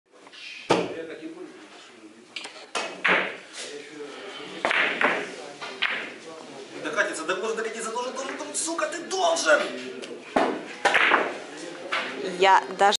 Случайный свой шар в угол накатом в исполнении Алексея
Пример нечаянного шара на турнире по свободной пирамиде, русский бильярд. Ролик содержит литературные ругательства, поэтому взрослым рекомендуется показывать этот фрагмент детям до 16 только без звука)